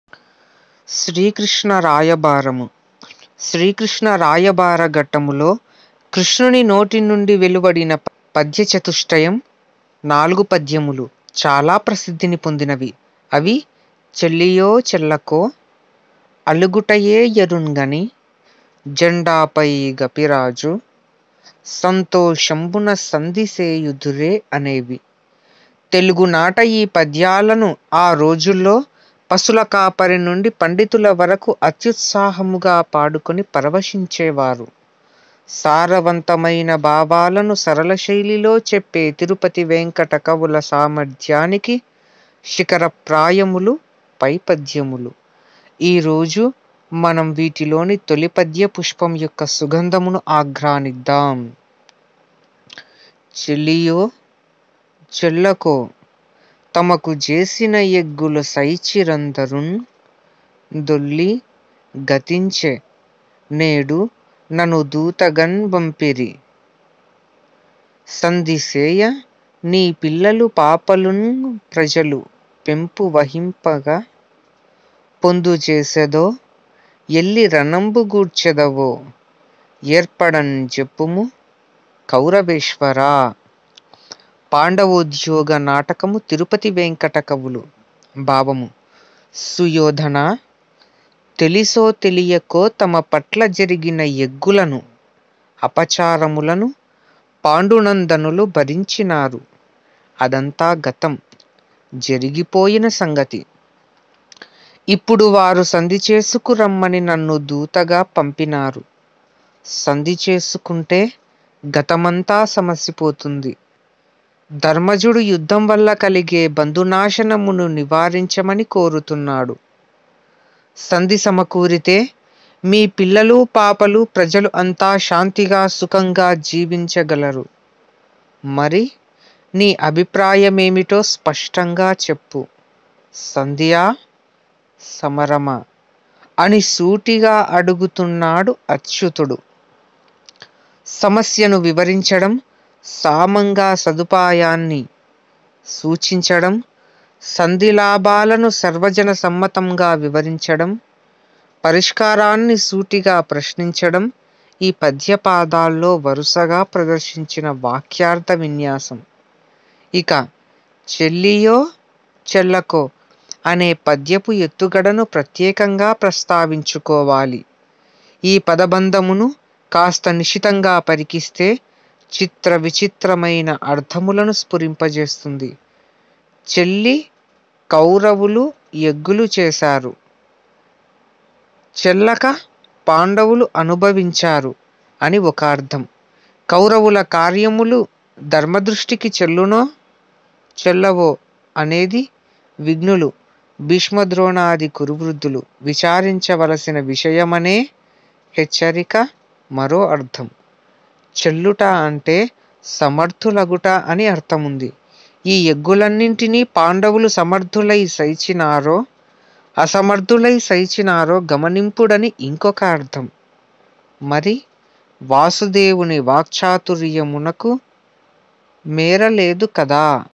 Sri Krushna Rayabharamu -1 || శ్రీ కృష్ణ రాయభారం
మనకు శుభమును చేకూర్చే ఏ విషయాన్నైన లెక్కచేయక ముందుకు వెళితే పర్యవసానాలు ఎలా ఉంటాయో తెలియజేసే పద్యం.!